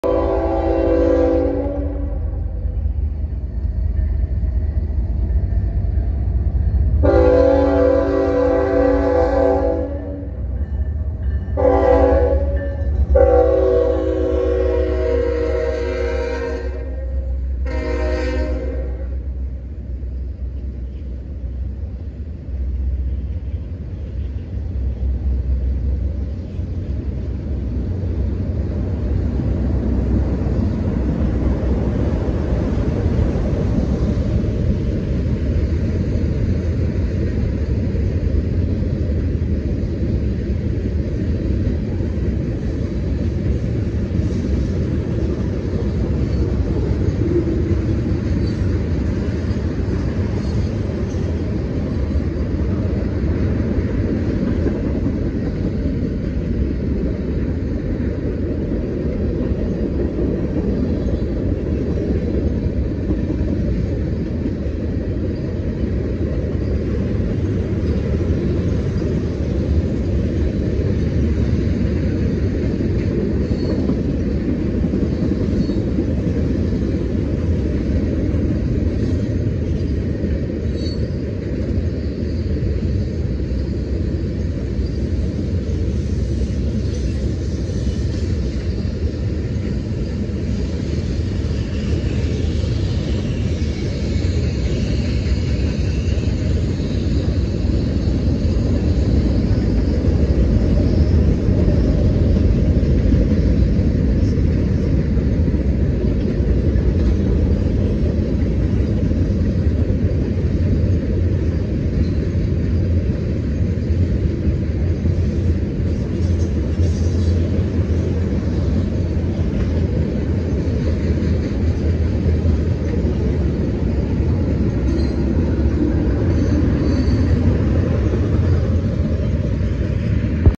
7-3-23: CN M396 in Centralia IL
CN freight train M396 heads north through Centralia IL on the CN Champaign sub